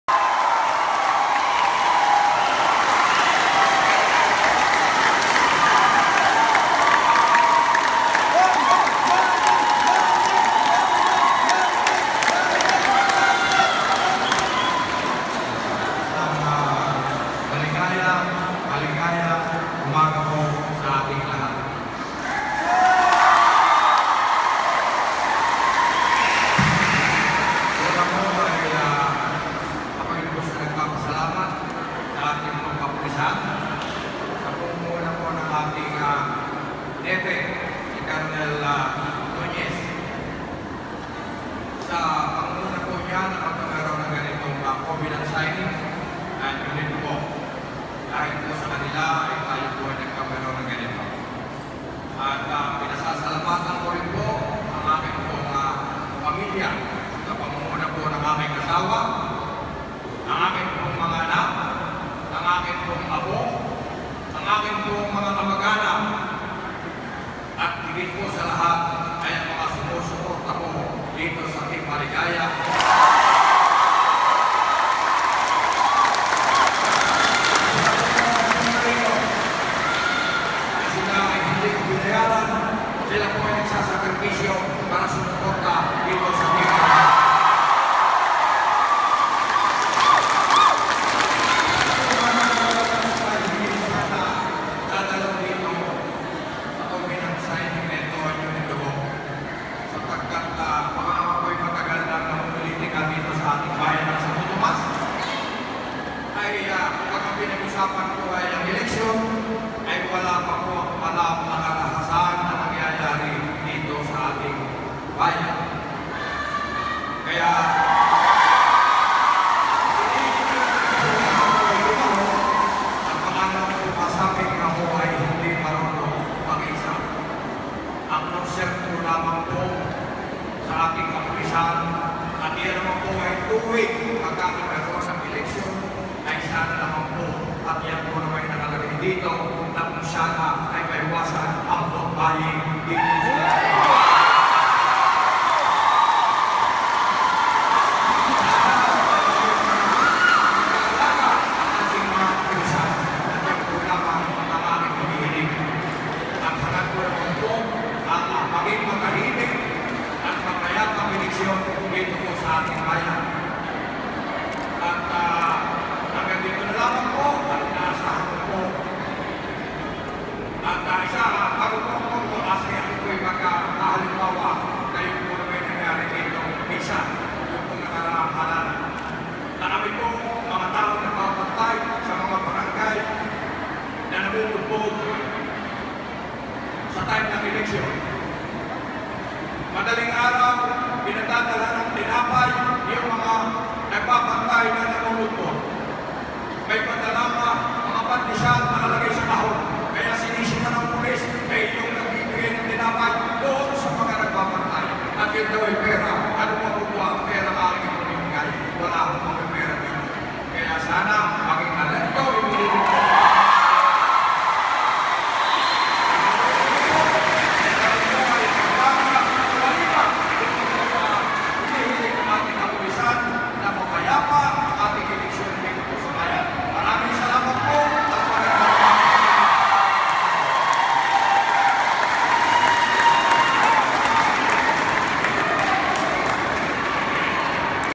Mayor Maligaya Covenant Speech
Peace Covenant. Secure & Fair Election 2016. No to VOTE BUYING!